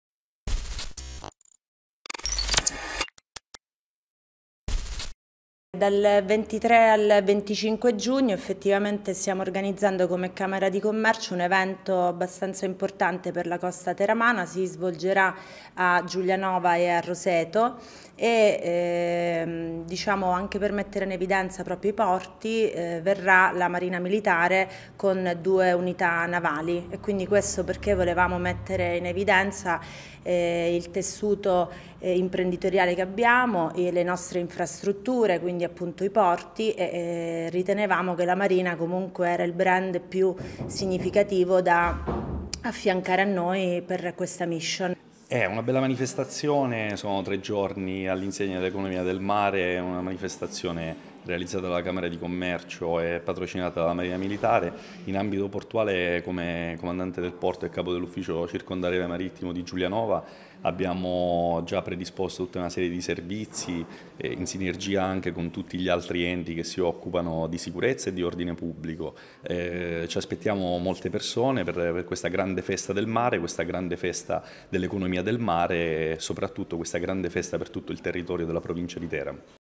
"TeraMare", dal 23 al 25 giugno mare protagonista tra Giulianova e Roseto
Tre giorni dedicati al mare e all’economia, alle attività connesse e alla valorizzazione del territorio costiero. “Teramare – La Costa Teramana protagonista della Blue Economy” in programma dal 23 al 25 giugno tra Giulianova e Roseto degli Abruzzi è l'evento ideato e promosso dalla Camera di Commercio Gran Sasso d'Italia